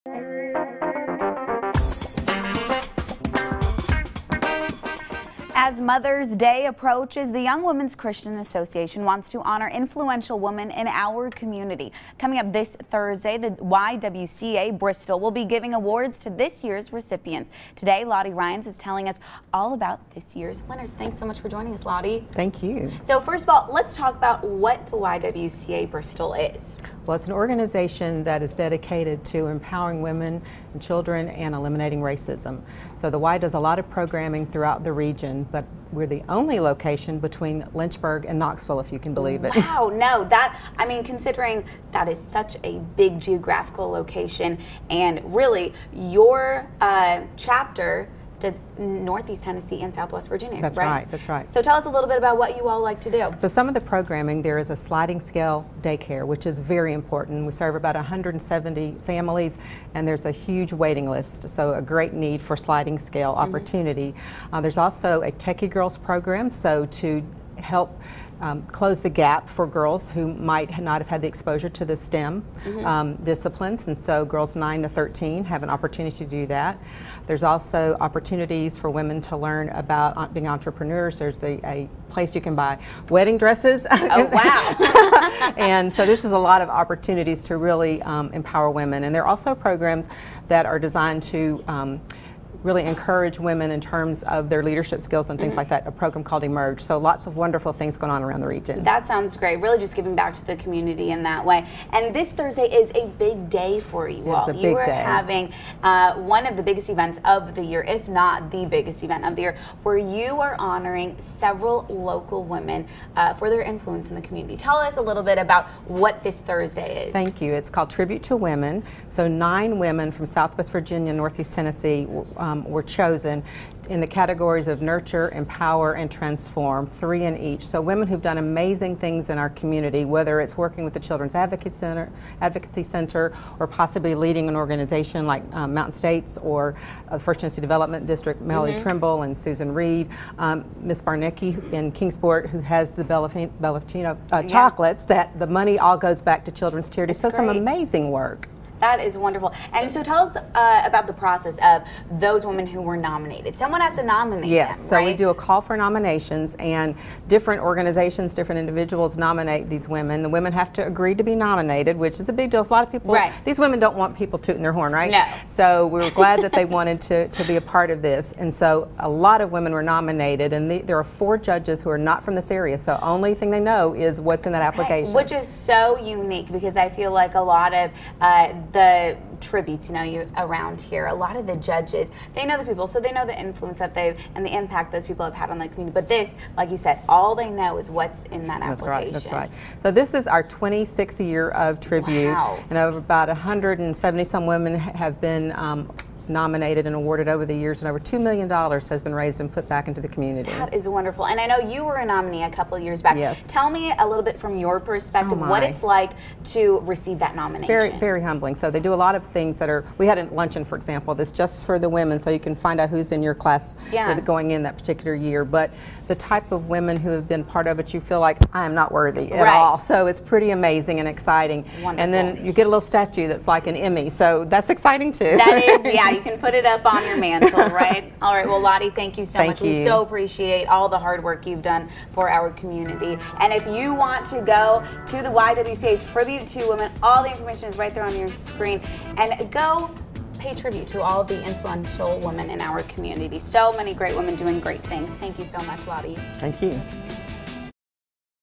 YWCA Tribute To Women
As Mother's Day approaches, the Young Women's Christian Association wants to honor influential women in our community! Coming up this Thursday, the YWCA Bristol will be giving awards to this year's recipients. This morning, Lottie Ryans is telling us more about this year's winners.